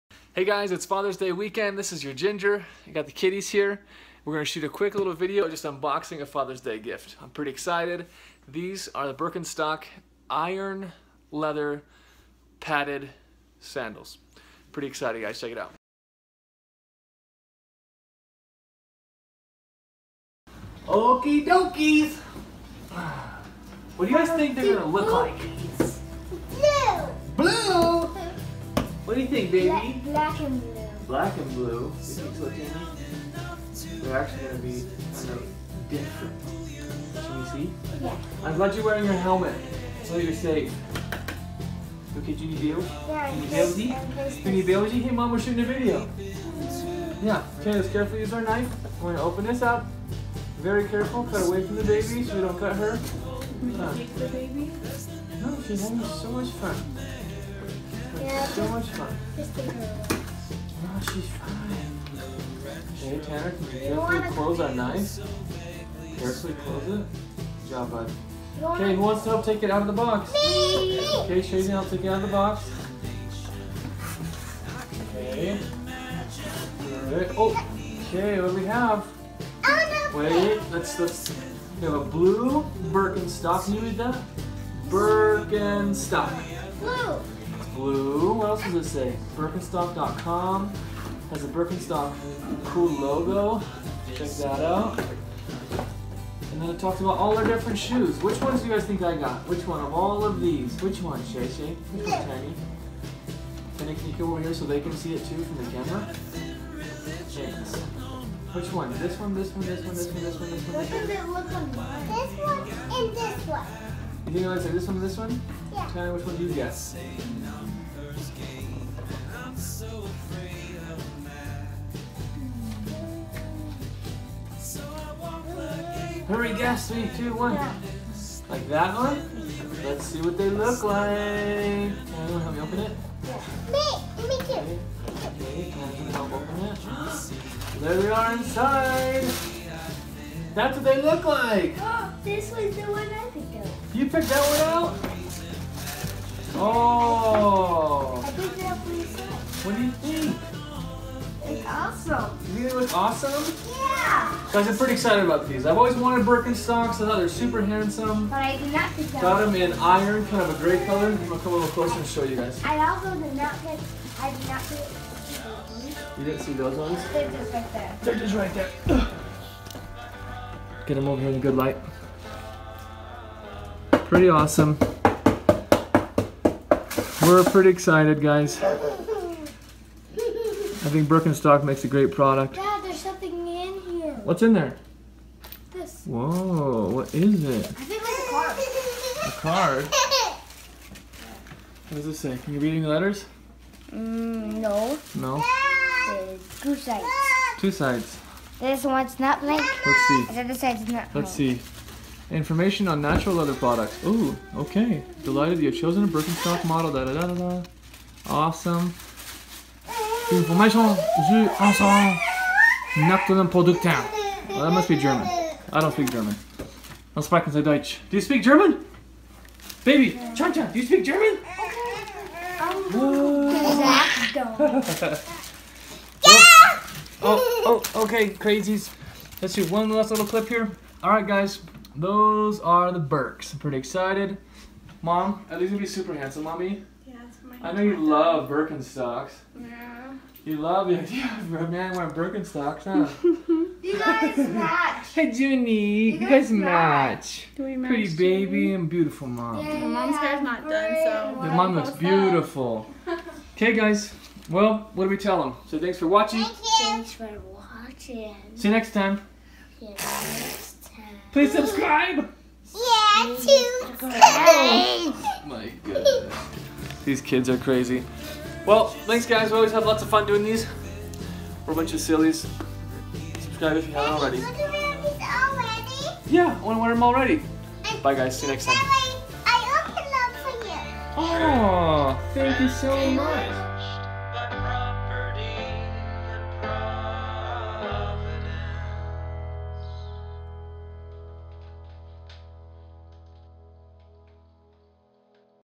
Birkenstock Iron Leather Unboxing | Best Birkenstocks
I have waited a long time to get my hands on a pair of Birkenstocks and after months of convincing my wife that they are the best DAD sandals out there we got a pair!

The kiddies and I had a blast opening them and per usual they were their crazy and goofy selves.

I had reviewed several different sites and ended up buying them

I ended up needing to customize them to make them fit my feet a little better and will be posting that video soon.

Thanks!